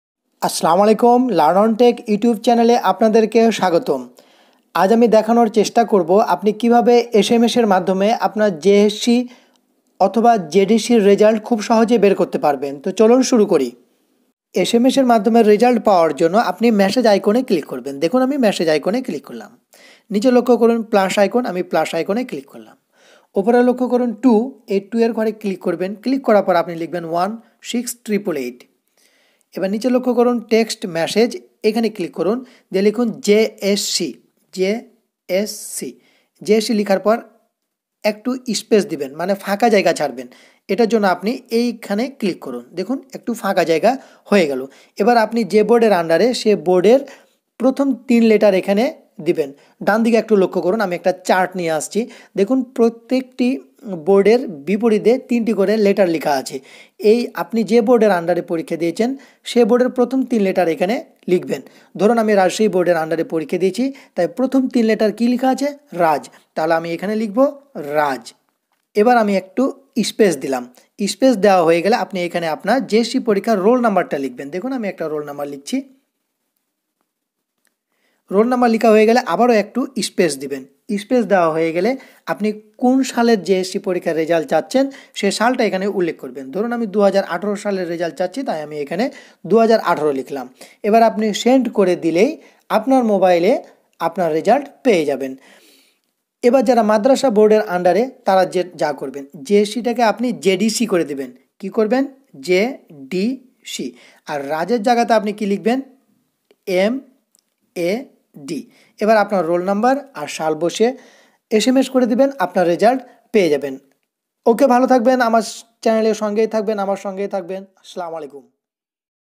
SMS-এর মাধ্যমে JSC/JDC Result দেখবেন যেভাবে।
Hello friends,
Good day! Hope you are all well. Here I'm trying to explain in depth about how to get your JSC/JDC Result via SMS.

My Target : Easy Tech tips for better life.